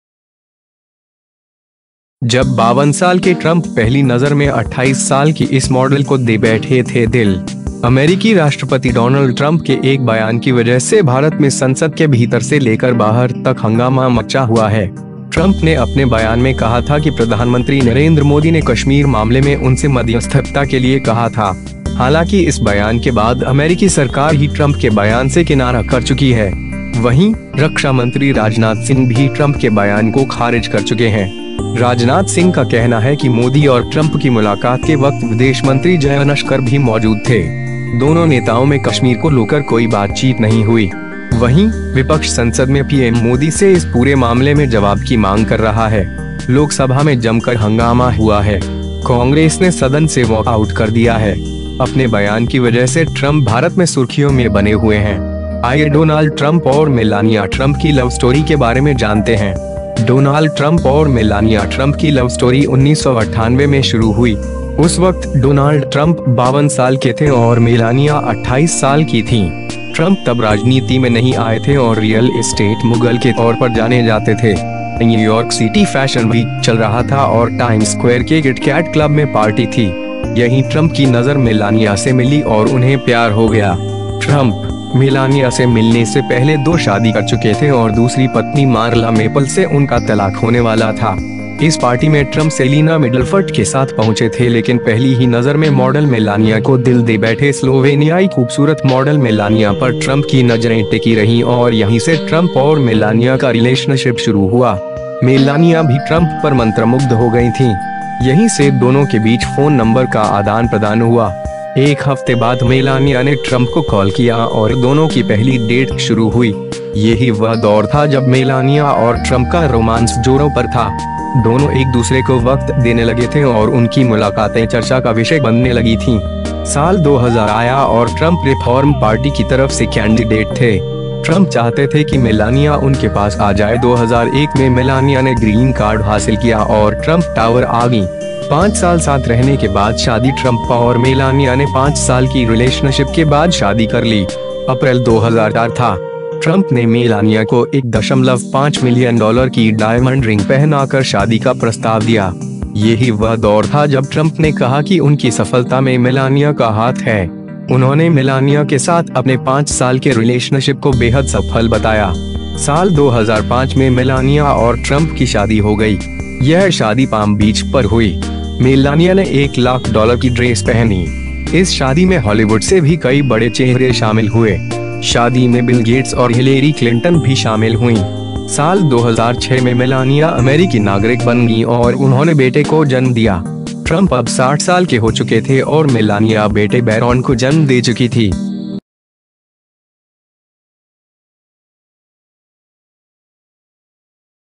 जब 52 साल के ट्रंप पहली नजर में 28 साल की इस मॉडल को दे बैठे थे दिल
for mouth watering Indian Food Recipes जब 52 साल के ट्रंप पहली नजर में 28 साल की इस मॉडल को दे बैठे थे दिल

अमेरिकी राष्ट्रपति डोनाल्ड ट्रंप के एक बयान की वजह से भारत में संसद के भीतर से लेकर बाहर तक हंगामा मचा हुआ है। ट्रंप ने अपने बयान में कहा था कि प्रधानमंत्री नरेंद्र मोदी ने कश्मीर मामले में उनसे मध्यस्थता के लिए कहा था। हालांकि इस बयान के बाद अमेरिकी सरकार ही ट्रंप के बयान से किनारा कर चुकी है। वहीं, रक्षा मंत्री राजनाथ सिंह भी ट्रंप के बयान को खारिज कर चुके हैं। राजनाथ सिंह का कहना है कि मोदी और ट्रंप की मुलाकात के वक्त विदेश मंत्री जयंशकर भी मौजूद थे। दोनों नेताओं में कश्मीर को लोकर कोई बातचीत नहीं हुई। वहीं, विपक्ष संसद में पीएम मोदी से इस पूरे मामले में जवाब की मांग कर रहा है। लोकसभा में जमकर हंगामा हुआ है। कांग्रेस ने सदन से वॉक आउट कर दिया है। अपने बयान की वजह से ट्रंप भारत में सुर्खियों में बने हुए हैं। आइए डोनाल्ड ट्रंप और मेलानिया ट्रंप की लव स्टोरी के बारे में जानते हैं...

डोनाल्ड ट्रंप और मेलानिया ट्रंप की लव स्टोरी 1998 में शुरू हुई। उस वक्त डोनाल्ड ट्रंप 52 साल के थे और मेलानिया 28 साल की थीं। ट्रंप तब राजनीति में नहीं आए थे और रियल-एस्टेट मुगल के तौर पर जाने जाते थे। न्यूयॉर्क सीटी फैशन वीक चल रहा था और टाइम्स स्क्वेयर के किट कैट क्लब में पार्टी थी। यहीं ट्रंप की नजर मेलानिया से मिली और उन्हें प्यार हो गया। ट्रंप, मेलानिया से मिलने से पहले दो शादी कर चुके थे और दूसरी पत्नी मारला मेपल से उनका तलाक होने वाला था।

इस पार्टी में ट्रंप सेलिना मिडलफ़र्ट के साथ पहुंचे थे लेकिन पहली ही नजर में मॉडल मेलानिया को दिल दे बैठे।स्लोवेनियाई खूबसूरत मॉडल मेलानिया पर ट्रंप की नजरें टिकी रहीं और यहीं से ट्रंप और मेलानिया का रिलेशनशिप शुरू हुआ। मेलानिया भी ट्रंप पर मंत्रमुग्ध हो गई थीं। यहीं से दोनों  के बीच फोन नंबर का आदान-प्रदान हुआ। एक हफ्ते बाद मेलानिया ने ट्रंप को कॉल किया और दोनों की पहली डेट शुरू हुई।

ये ही वह दौर था जब मेलानिया और ट्रंप का रोमांस जोरों पर था। दोनों एक-दूसरे को वक्त देने लगे थे और उनकी मुलाकातें चर्चा का विषय बनने लगी थीं। साल 2000 आया और ट्रंप रिफॉर्म पार्टी की तरफ से कैंडिडेट थे। ट्रंप चाहते थे कि मेलानिया उनके पास आ जाए। 2001 में मेलानिया ने ग्रीन कार्ड हासिल किया और ट्रंप टावर आ गईं।

पांच साल साथ रहने के बाद शादी
ट्रंप और मेलानिया ने पांच साल की रिलेशनशिप के बाद शादी कर ली। अप्रैल 2004 था। ट्रंप ने मेलानिया को 1.5 मिलियन डॉलर की डायमंड रिंग पहनाकर शादी का प्रस्ताव दिया। ये ही वह दौर था जब ट्रंप ने कहा कि उनकी सफलता में मेलानिया का हाथ है। उन्होंने मेलानिया के साथ अपने पांच साल के रिलेशनशिप को बेहद सफल बताया।

साल 2005 में मेलानिया और ट्रंप की शादी हो गई। यह शादी पाम बीच पर हुई। मेलानिया ने एक लाख डॉलर की ड्रेस पहनी। इस शादी में हॉलीवुड से भी कई बड़े चेहरे शामिल हुए। शादी में बिल गेट्स और हिलेरी क्लिंटन भी शामिल हुईं। साल 2006 में मेलानिया अमेरिकी नागरिक बन गईं और उन्होंने बेटे को जन्म दिया। ट्रंप अब 60 साल के हो चुके थे और मेलानिया बेटे बैरॉन को जन्म दे चुकी थी।